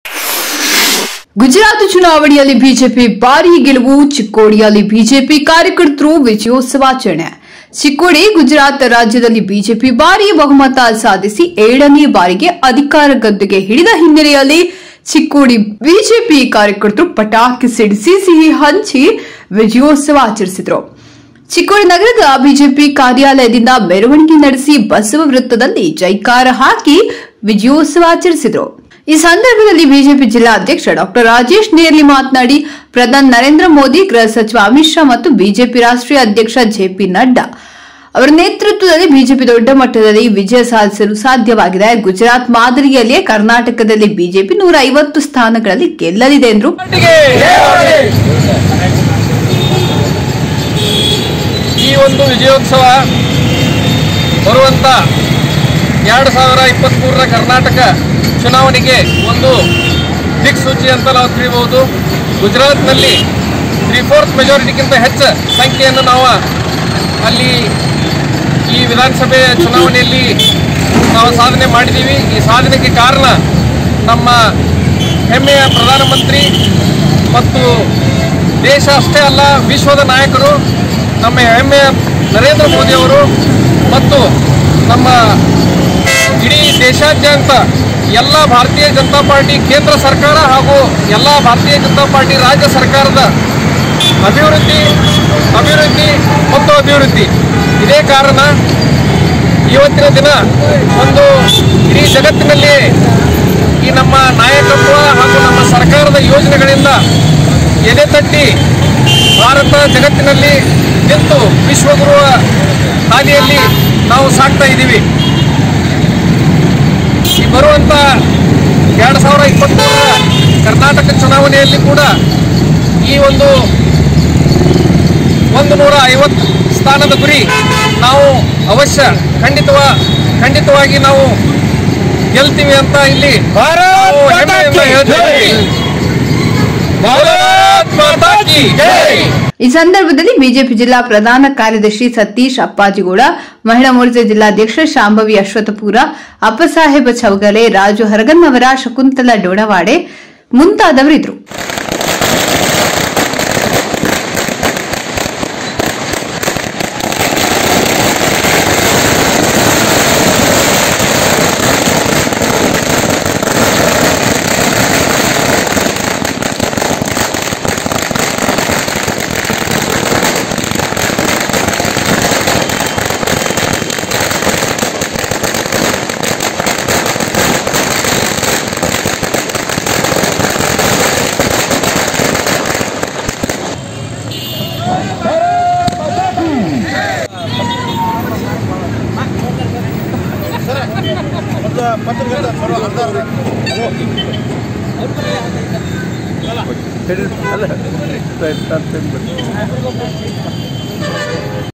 ಗುಜರಾತು ಚುನಾವಣೆಯಲ್ಲಿ ಬಿಜೆಪಿ ಭಾರಿ ಗೆಲುವು : ಚಿಕ್ಕೋಡಿಯಲ್ಲಿ ಬಿಜೆಪಿ ಕಾರ್ಯಕರ್ತರು ವಿಜಯೋತ್ಸವ ಆಚರಣೆ
ಗುಜರಾತು ಚುನಾವಣೆಯಲ್ಲಿ ಬಿಜೆಪಿ ಭಾರಿ ಗೆಲುವು : ಚಿಕ್ಕೋಡಿಯಲ್ಲಿ ಬಿಜೆಪಿ ಕಾರ್ಯಕರ್ತರು ವಿಜಯೋತ್ಸವ ಆಚರಣೆ (ICB NEWS CHIKODI)